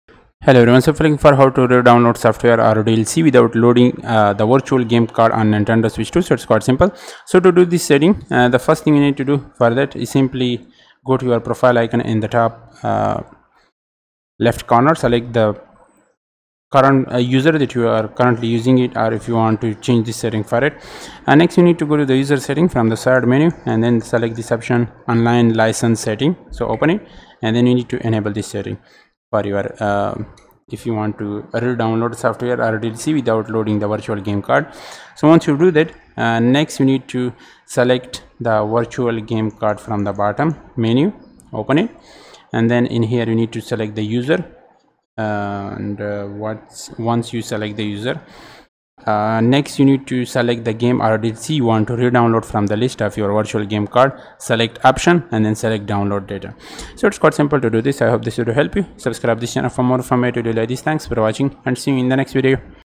How to redownload software or DLC without loading the Virtual game card in Nintendo Switch 2
In this video, you will learn How to redownload software or DLC without loading the Virtual game card in Nintendo Switch 2.